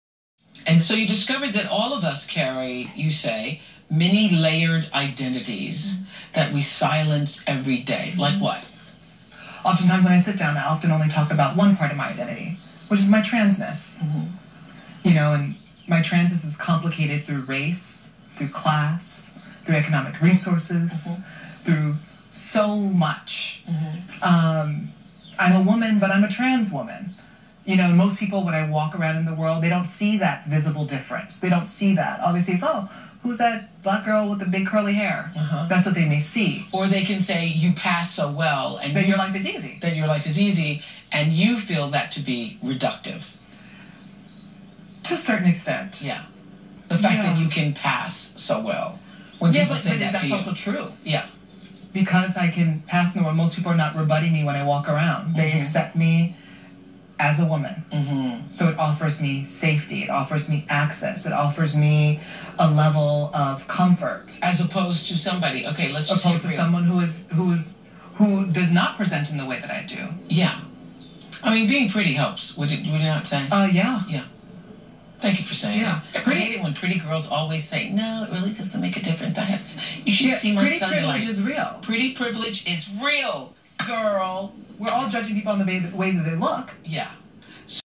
Janet Mock on Being Judge By Our Looks(1)
Janet Mock on Layered Identities Judgement  & Pretty Privileged